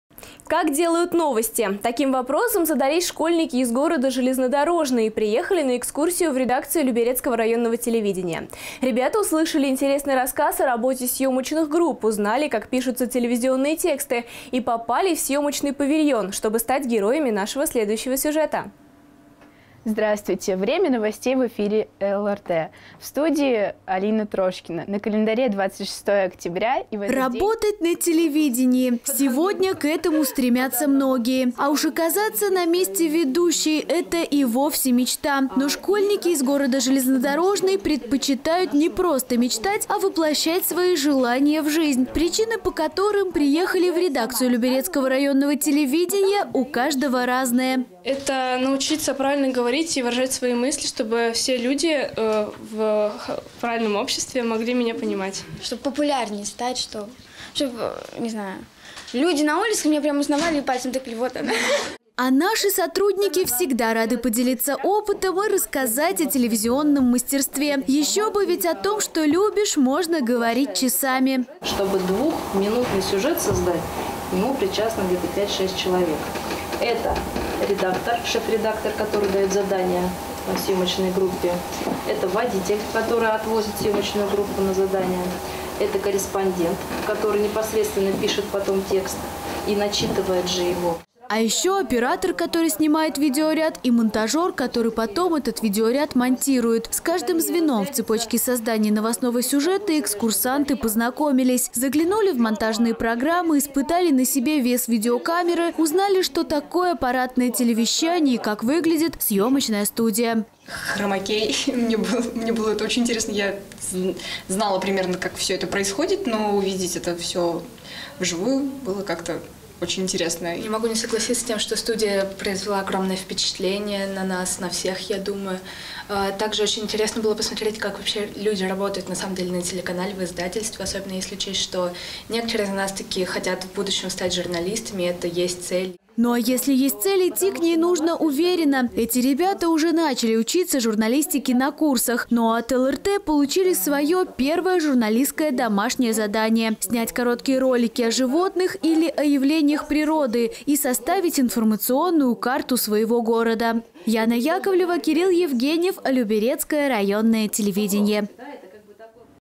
Телеканал ЛРТ провел экскурсию для школьников
«Как делают новости?» таким вопросом задались школьники из города Железнодорожный. И приехали на экскурсию в редакцию Люберецкого районного телевидения. Ребята услышали интересный рассказ о работе съемочных групп, узнали, как пишутся телевизионные тексты, попали в съемочный павильон и стали героями сюжета.

Работать на телевидении  - сегодня к этому стремятся многие. А уж оказаться на месте ведущей – это и вовсе мечта. Но школьники из города Железнодорожный предпочитают не просто мечтать, а воплощать свои желания в жизнь.  Причины, по которым приехали  в редакцию Люберецкого районного телевидения, у каждого разные.

Вероника Марцинкевич, ученица гимназии №9,  г. Железнодорожный:

«Научиться правильно говорить и выражать свои мысли, чтобы все люди в правильном обществе могли меня понимать».

Анна Петросян, ученица гимназии №9,  г. Железнодорожный:

«Чтобы популярнее стать. Чтоб люди на улицах меня узнавали и пальцем тыкали: «Вот она!»».

А наши сотрудники всегда рады поделиться опытом и рассказать о телевизионном мастерстве. Еще бы, ведь о том, что любишь, можно говорить часами.

Екатерина Дегтярева, генеральный директор ООО «ЛРТ»:

«Чтобы 2-х минутный сюжет создать, к нему причастны где-то 5-6 человек. Это редактор, шеф-редактор, который дает задание съемочной группе. Это водитель, который отвозит съемочную группу на задание, это корреспондент, который непосредственно пишет потом текст. И начитывает же его».

А еще оператор, который снимает видеоряд, и монтажер, который потом этот видеоряд монтирует. С каждым звеном в цепочки создания новостного сюжета экскурсанты познакомились. Заглянули в монтажные программы, испытали на себе вес видеокамеры, узнали что такое аппаратная телевещания и как выглядит съемочная студия.

Надежда Асерчева, ученица гимназии №9,  г. Железнодорожный:

«Хромакей. Мне было это очень интересно, я знала примерно, как все это происходит, но увидеть это все в живую - было как-то очень интересно».

Наталия Батарагина, ученица гимназии №9,  г. Железнодорожный:

«Студия произвела огромное впечатление на нас на всех, я думаю. Так же было очень интересно посмотреть, как люди работают на самом деле, на телеканале, в издательстве. Особенно, если учесть, что некоторые из нас таки хотят в будущем стать журналистами, это есть цель».

Ну, а если есть цель – идти к ней нужно уверено. Эти ребята уже начали учиться журналистике на курсах, ну а от ЛРТ получили свое первое журналистское домашнее задание: снять короткие ролики о животных или о явлениях природы и составить информационную карту своего города.

Яна Яковлева, Кирилл Евгеньев